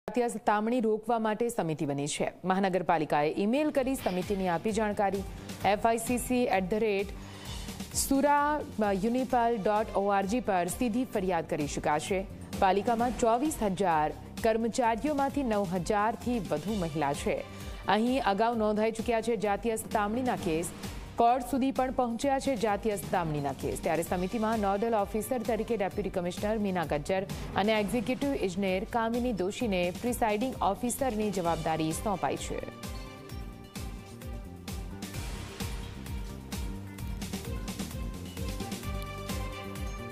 Surat : SMC જાતીય સતામણી રોકવા બની સમિતિ : E-Mail કરી સમિતિની આપી જાણકારી
સુરત મહાનગરપાલિકામાં જાતીય સતામણી રોકવા બની સમિતિ : ઈમેઈલ કરી સમિતિની આપી જાણકારી